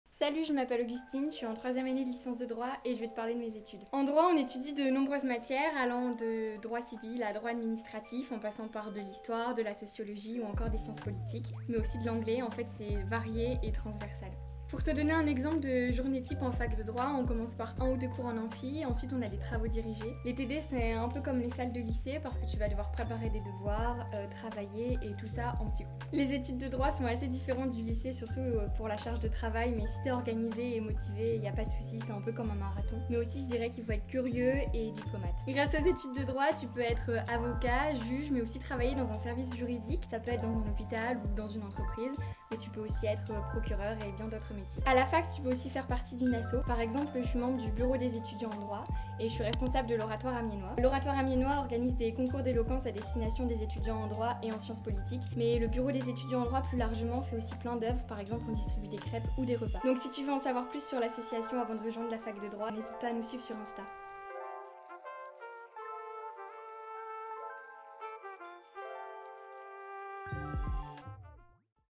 Augustine, étudiante à l'UFR de Droit
"Les études de droit [...] c'est un peu comme un marathon !" 🏃

🎥 Augustine, étudiante à l'UPJV, te présente la licence de droit. Elle est aussi membre d'une association étudiante : le bureau des étudiants en droit !